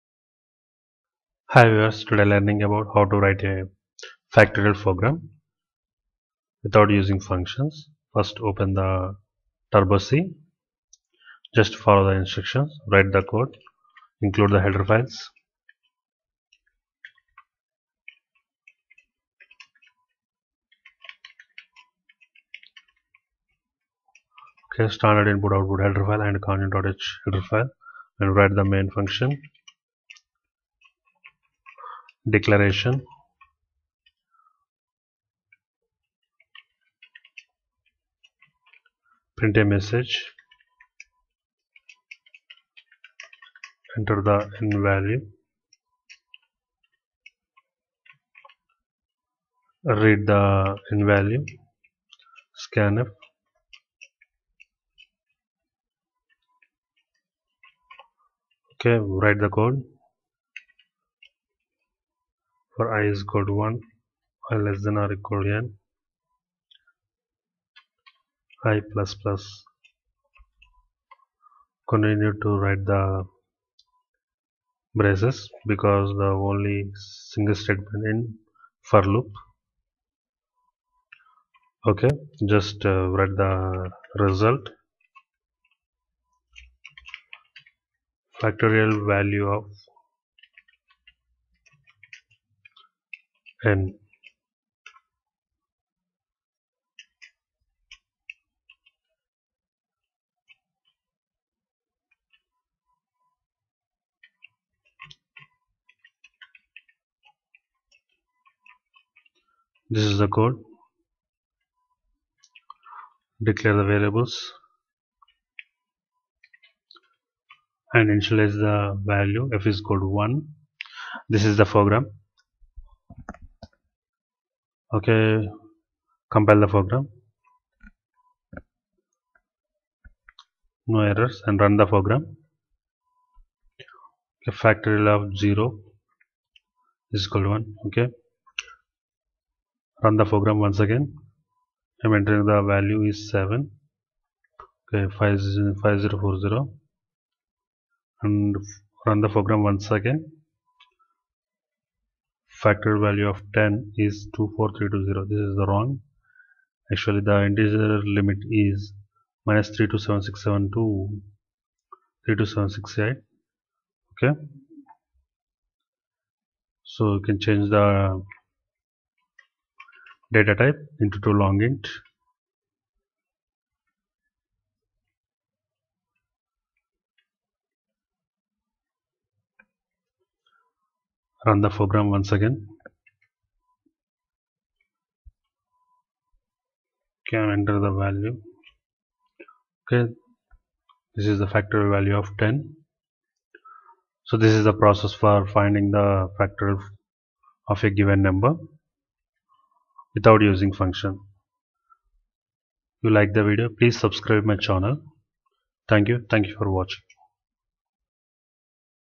C Program For Factorial Of a Number - Factorial C Program Without Using Functions
In this Video Factorial of a Given Number

Factorial Program Without Using Functions

C Program For Factorial of a Given Number